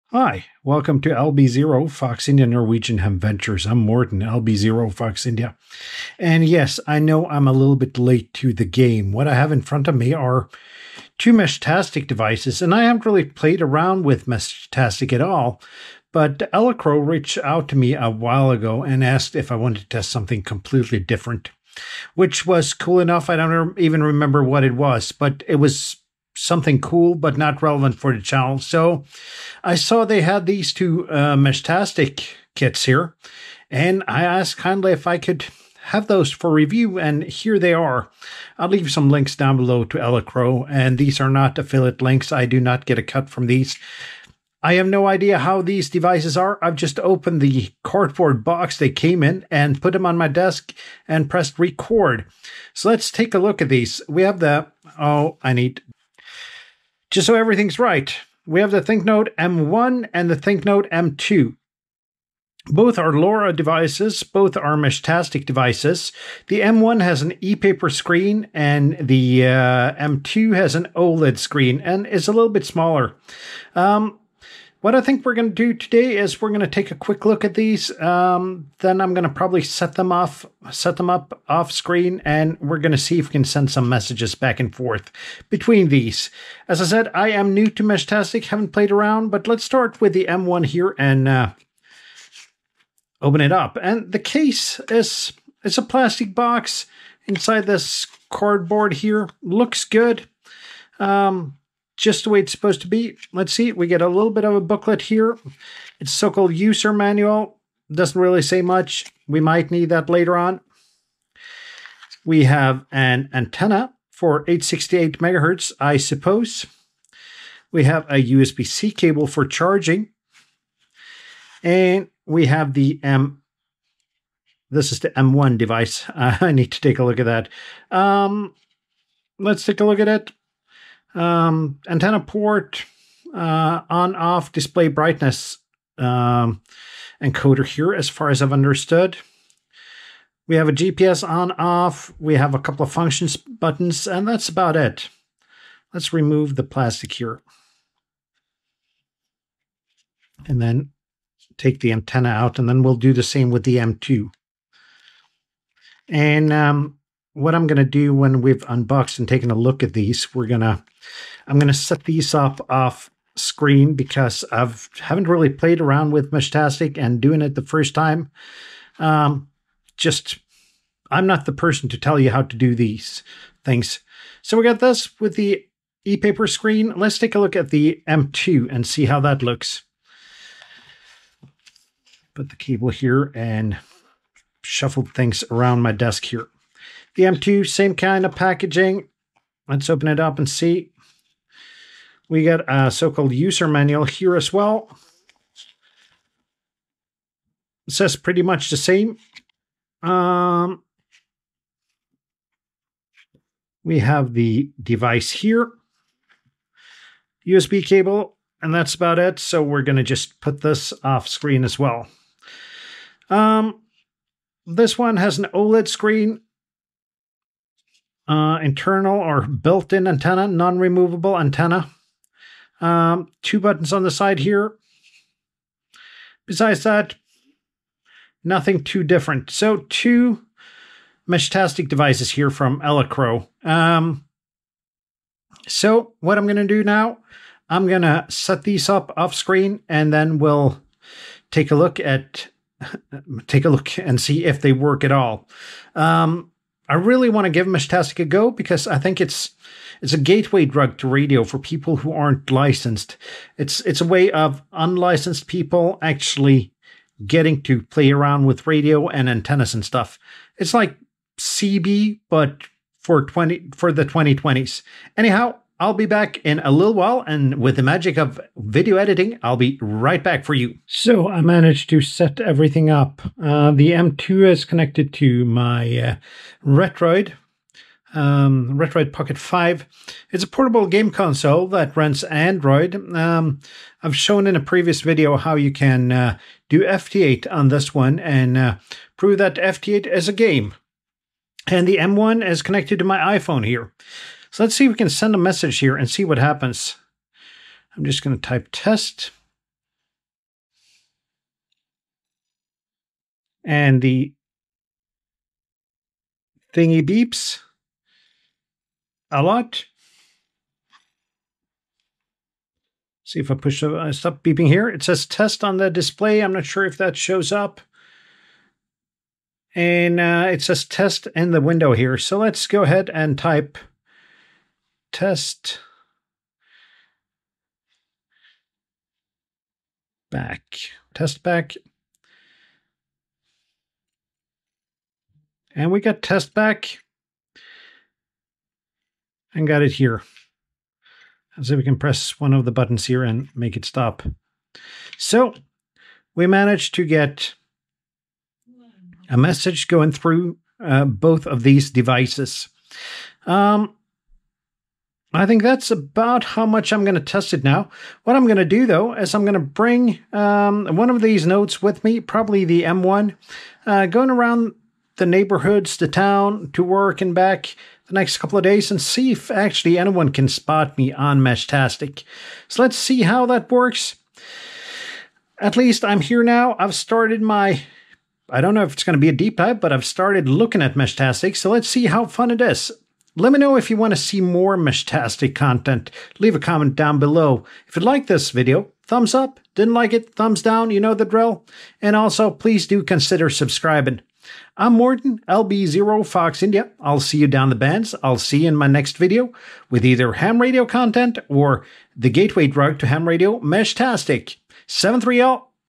A first look at the Elecrow ThinkNode M1 and M2
Yes I know I am late to the game, and I wasn't even thinking about doing videos on Meshtastic until Elecrow reached out and asked if I wanted to take a look at the ThinkNode M1 and the ThinkNode M2. This is not a review, but a first look at the devices.

You can find the M1 and the M2 here:
ThinkNode M1 LoRa Meshtastic Transceiver